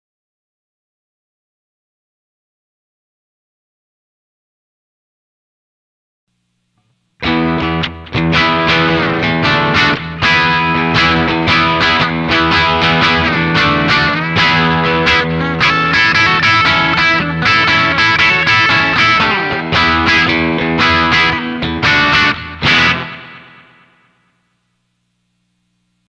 P90 BRIDGE FULL
Description The sound of a (FULL) P90 pickup in the bridge position of a Les Paul.